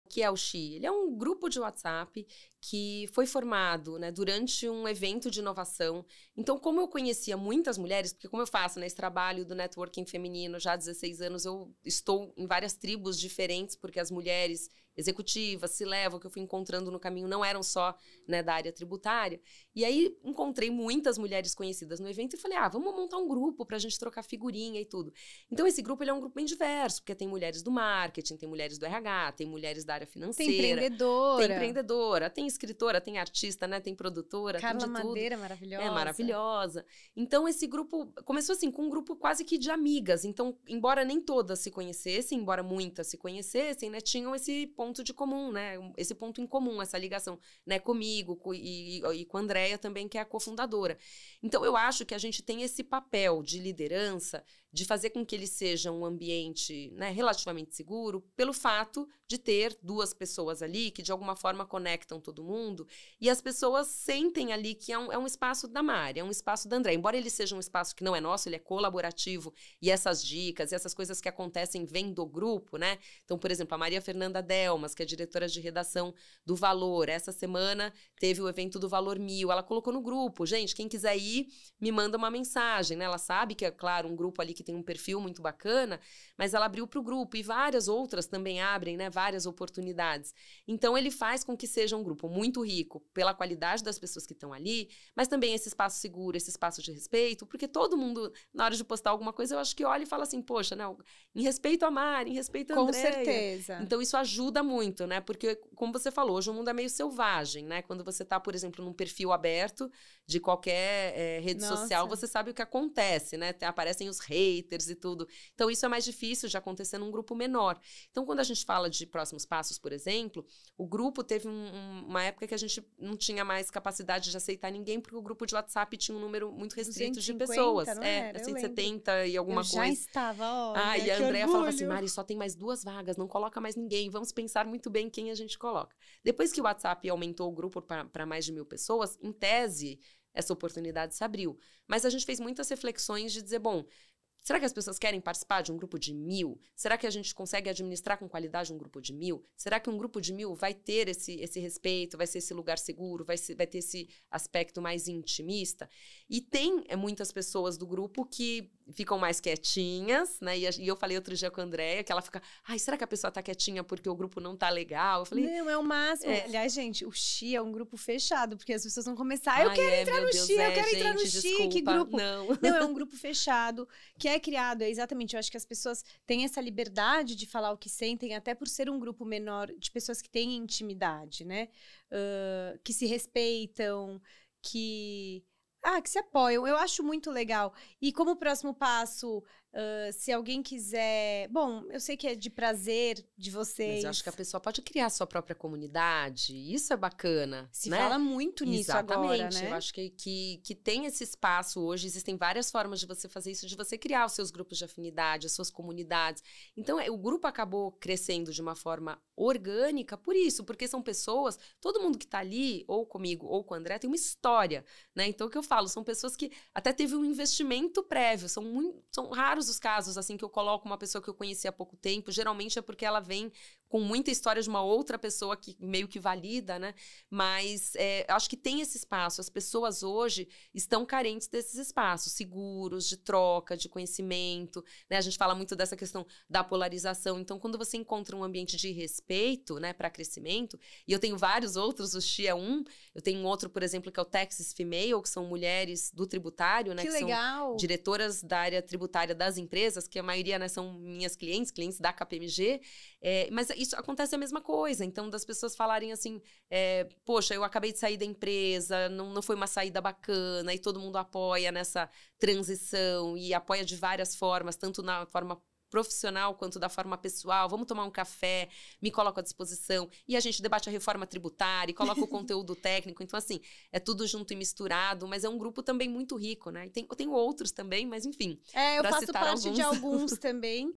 Como criar um grupo de afinidade e networking para liderança feminina? | Marienne Coutinho corte 3
A Marienne Coutinho, sócia da KPMG e fomentadora do networking e liderança feminina, usa de várias frentes para promover e conectar mulheres, uma delas é simples e funcional: o Whatsapp. O SHE é um grupo de afinidade e liderança feminina que conta com mulheres de diversas áreas.
O espaço é colaborativo, mas a meta é única: conversar sobre as mulheres e o feminino no mercado de trabalho, em um espaço seguro e intimista.